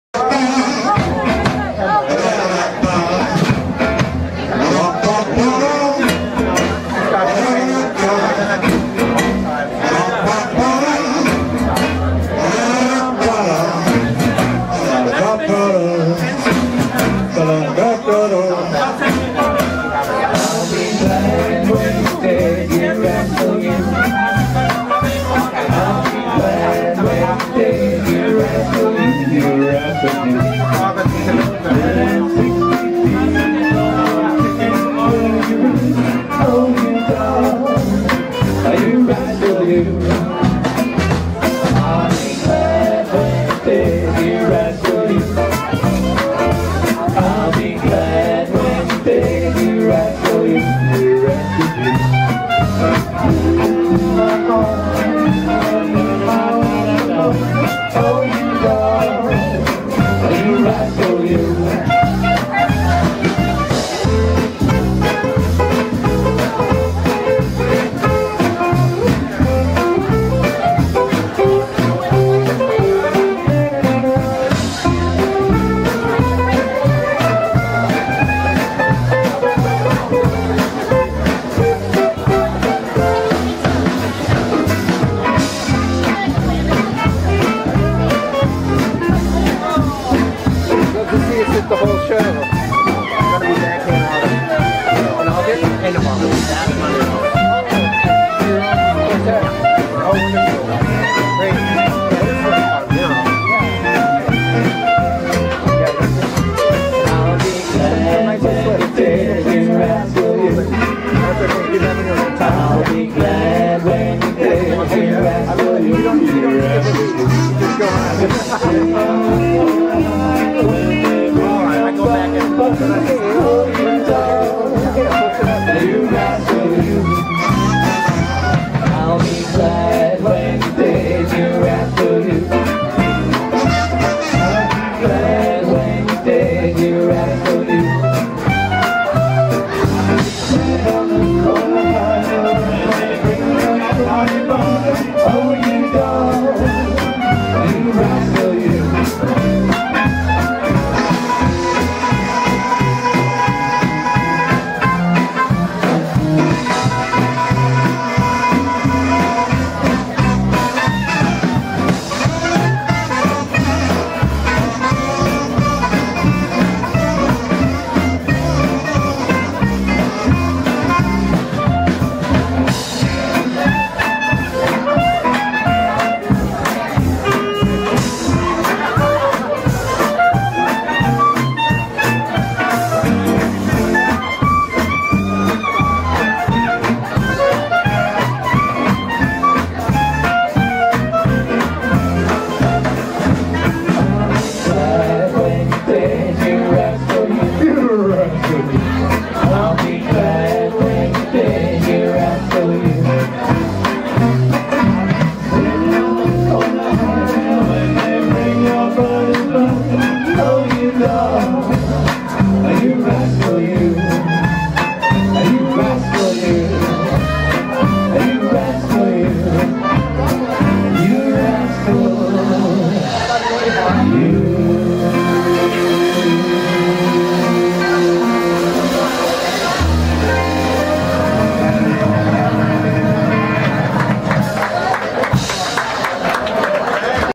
Icy Hot Club of California feat. Chloe Feoranzo - 5.22.12 805
Icy Hot Club of California at Joe's - 5.22.12

Featuring Chloe Feoranzo on clarinet
BlackJack LaChance - Contrebasse et vocals
Ray Bergstrom - guitar et vocals
Ethan Emerson - guitar et vocals
Mario Montes - drums

Chloe Feoranzo